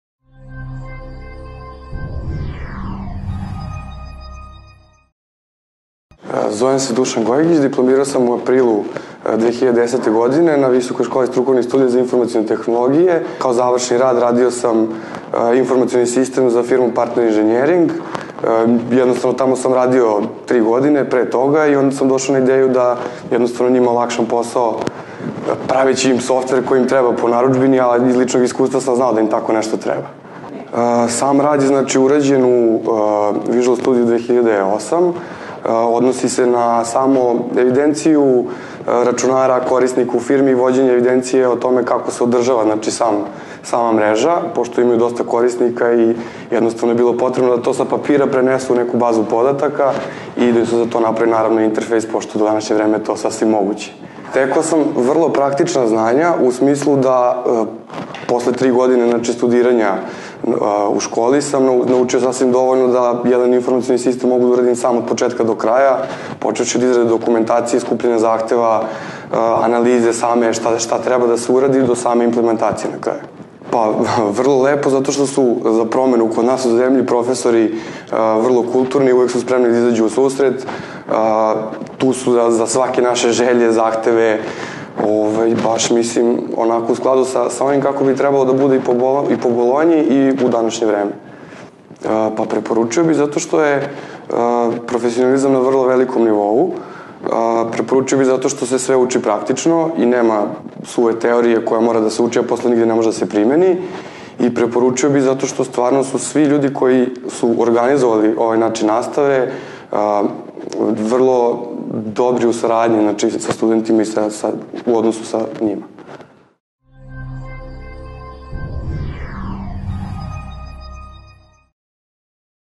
ITS diplomac - Dušan Gojagić
Diplomac Visoke škole strukovnih studija za informacione tehnoogije - ITS iznosi utiske o studijama i stečenom znanju.